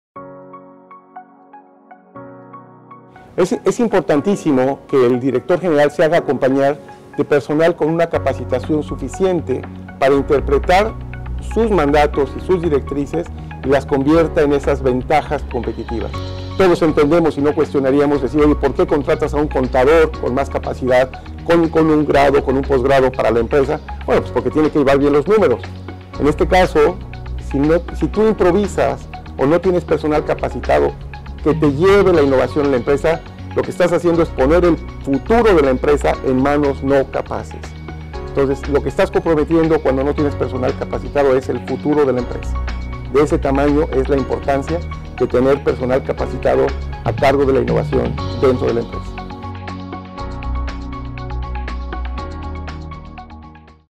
¿Por qué es importante capacitarse en Innovación? | Posgrado Anáhuac Mayab
Capacítate en Innovación. Conoce los consejos que nos comparte el Dr. Guillermo Aguirre.